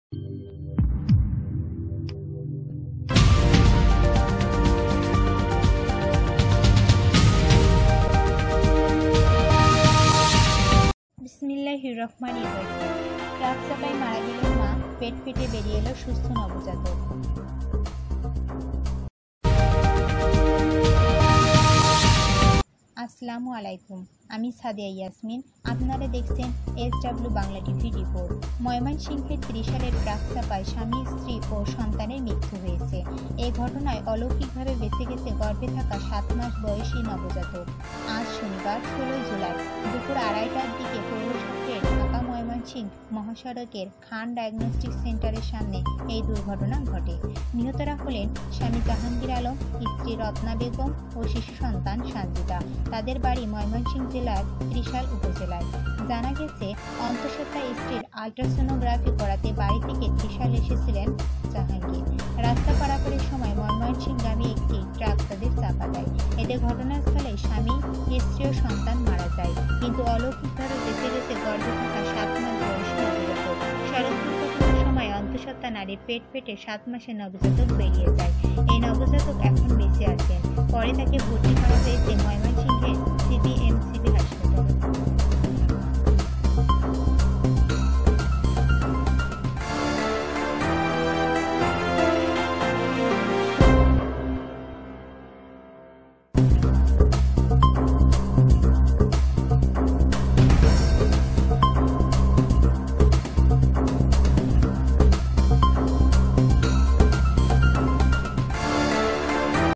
ট্রাকচাপায় মারা গেলেন মা, পেট ফেটে বেরিয়ে এলো সুস্থ নবজাতক।এস,ডব্লিউ বাংলা টিভি।
ট্রাকচাপায় মারা গেলেন মা, পেট ফেটে বেরিয়ে এলো সুস্থ নবজাতক। .

  আসসালামু আলাইকুম আপনারা দেখছেন এস,ডব্লিউ বাংলা টিভির  রিপোর্ট,
ময়মনসিংহের ত্রিশালে ট্রাকচাপায় স্বামী-স্ত্রী ও সন্তানের মৃত্যু হয়েছে। এ ঘটনায় অলৌকিকভাবে বেঁচে গেছে গর্ভে থাকা সাত মাস বয়সী নবজাতক।

আজ শনিবার (১৬ জুলাই) দুপুর ২.৩০ টার দিকে পৌর শহরের ঢাকা-ময়মনসিংহ মহাসড়কের খান ডায়াগনস্টিক সেন্টারের সামনে এ দুর্ঘটনা ঘটে।

নিহতরা হলেন, স্বামী জাহাঙ্গীর আলম, স্ত্রী রত্না বেগম ও শিশু সন্তান সানজিদা। তাদের বাড়ি ময়মনসিংহ জেলার ত্রিশাল উপজেলায়।

জানা গেছে, অন্তঃসত্ত্বা স্ত্রীর আলট্রাসনোগ্রাফি করাতে বাড়ি থেকে ত্রিশাল এসেছিলেন জাহাঙ্গীর। রাস্তা পারাপারের সময় ময়মনসিংহ গামী একটি ট্রাক তাদের চাপা দেয়। এতে ঘটনাস্থলেই স্বামী, স্ত্রী ও সন্তান মারা যায়। কিন্তু অলৌকিকভাবে বেঁচে গেছে গর্ভে থাকা সাত মাস বয়সী নবজাতক। সড়ক দুর্ঘটনার সময় অন্তঃসত্ত্বা নারীর পেট ফেটে সাত মাসের নবজাতক বেরিয়ে যায়।

এই নবজাতক এখন বেঁচে আছেন। পরে তাকে ভর্তি করা হয়েছে ময়মনসিংহের সিবিএমসিবি হাসপাতালে।